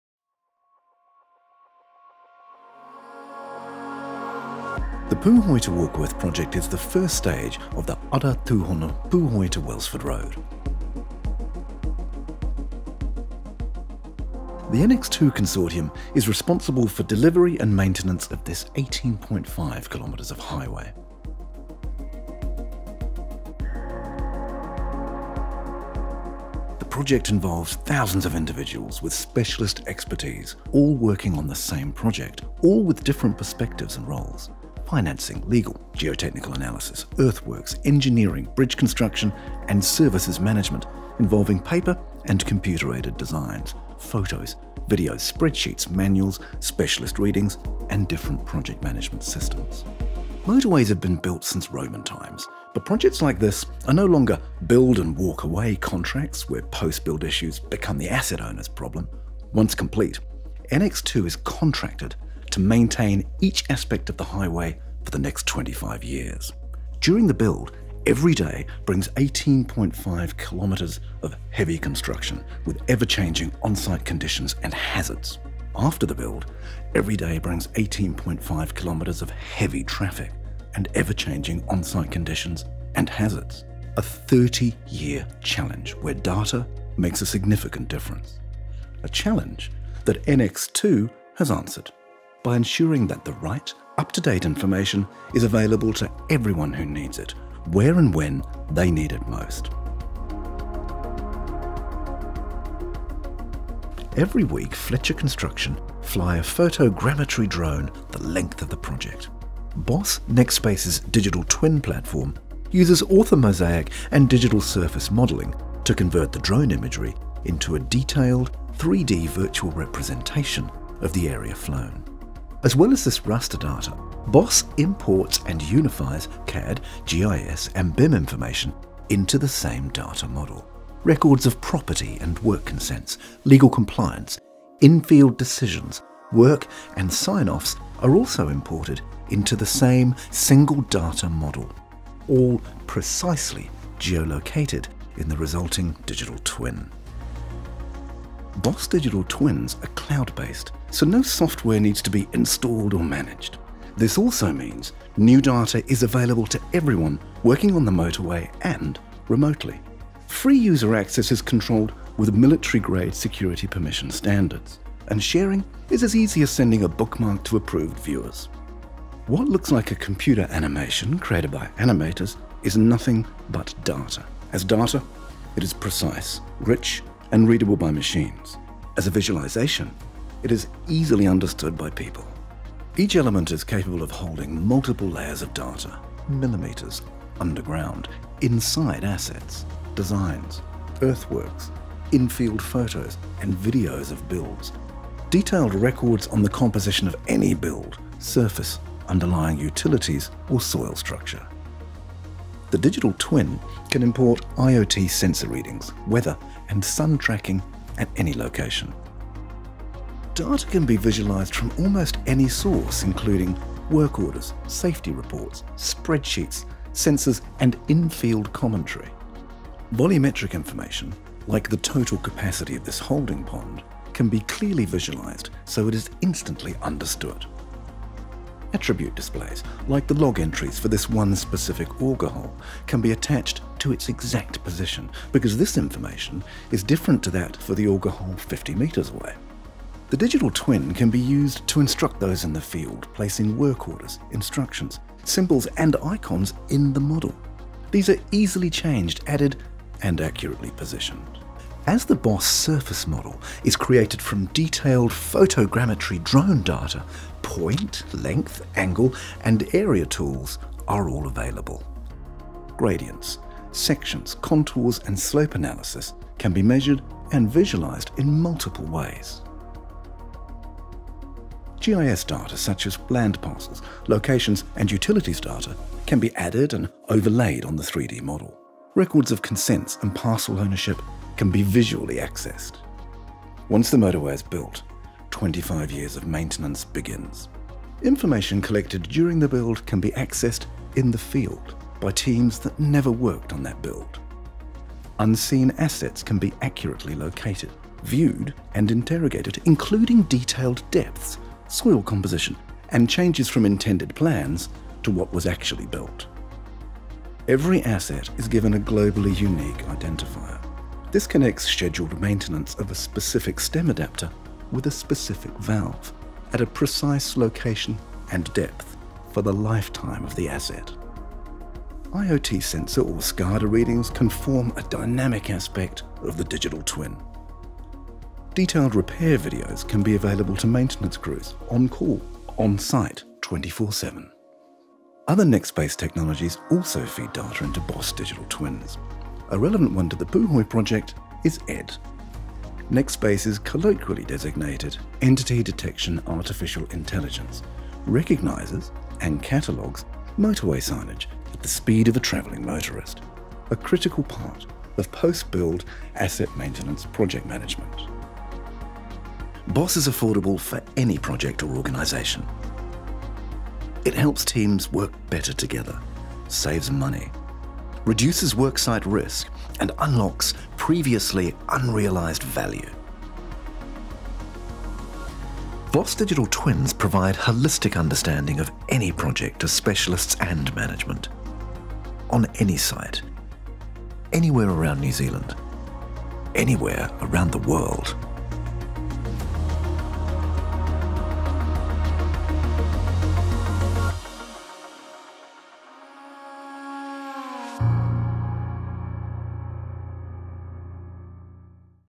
Showcase - Puhoi Warkworth Motorway Extension - Nextspace Digital Twin Platform
Join us to see how data from dozens of different sources across 11.5 miles of highway is brought together, visualized and continuously updated in a Nextspace digital twin across the build and maintenance lifestages of this highway project in New Zealand's North Island.

For more on the Nextspace digital twin platform, head to our

AUTO-GENERATED TRANSCRIPT
The Pūhoi to Warkworth project is the first stage of the Ara Tūhono Pūhoi to Wellsford road. The NX2 Consortium is responsible for the delivery and maintenance of this 18.5 kilometers of highway. The project involves thousands of individuals with specialist expertise all working on the same project, all with different perspectives and roles—financing, legal, geotechnical analysis, earthworks, engineering, bridge construction and services management— involving paper and computer-aided designs, photos, video, spreadsheets, manuals, specialist readings and different project management systems. Motorways have been built since Roman times, but projects like this are no longer ‘build-and-walk-away’ contracts where post-build issues become the asset owners’ problem once complete. NX2 is contracted to maintain each aspect of the highway for the next 25 years during the build. Every day brings 18.5 kilometers of heavy construction with ever-changing on-site conditions and hazards. After the build, every day brings 18.5 kilometers of heavy traffic and ever-changing on-site conditions and hazards. A 30-year challenge where data makes a significant difference. A challenge that NX2 has answered by ensuring that the right up-to-date information is available to everyone who needs it, where and when they need it. Almost every week, Fletcher Construction fly a photogrammetry drone the length of the project. Nextspace's digital twin platform uses author mosaic and digital surface modeling to convert the drone imagery into a detailed 3D virtual representation of the area flown, as well as this raster data, Nextspace imports and unifies CAD, GIS and BIM information into the same data model records of property and work consents, legal compliance, in-field decisions. Work and sign-offs are also imported into the same single data model—all precisely geo-located in the resulting digital twin. Nextspace digital twins are cloud-based, so no software needs to be installed or managed. This also means new data is available to everyone working on the motorway, and remotely. Free user access is controlled with military-grade security permission standards and sharing is as easy as sending a bookmark to approved viewers. What looks like a computer animation created by animators is nothing but data, as data it is precise, rich and readable by machines. As a visualization, it is easily understood by people. Each element is capable of holding multiple layers of data—millimeters underground, inside assets, designs, earthworks, in-field photos and videos of builds. Detailed records on the composition of any build surface, underlying utilities or soil structure. The digital twin can import IoT sensor readings, weather and sun tracking at any location. Data can be visualized from almost any source, including work orders, safety reports, spreadsheets, sensors, and in-field commentary. Volumetric information like the total capacity of this holding pond can be clearly visualized, so it is instantly understood. Attribute displays like the log entries for this one specific orga hole can be attached to its exact position because this information is different to that for the orga hole 50 meters away. The digital twin can be used to instruct those in the field, placing work orders, instructions, symbols and icons in the model. These are easily changed, added, and accurately positioned as the Nextspace surface model is created from detailed photogrammetry, drone data, point length, angle and area tools are all available. Gradients, sections, contours and slope analysis can be measured and visualized in multiple ways. GIS data such as land parcels, locations and utilities data can be added and overlaid on the 3D model. Records of consent and parcel ownership can be visually accessed. Once the motorway is built, 25 years of maintenance begins. Information collected during the build can be accessed in the field by teams that never worked on that build. Unseen assets can be accurately located, viewed and interrogated, including detailed depths, soil composition, and changes from intended plans to what was actually built. Every asset is given a globally unique identifier—this connects scheduled maintenance of a specific stem adapter with a specific valve at a precise location and depth for the lifetime of the asset. IoT sensor or SCADA readings can form a dynamic aspect of the digital twin. Detailed repair videos can be available to maintenance crews, on call on site 24/7. Other Nextspace technologies also feed data into Nextspace digital twins. [TRUNCATED]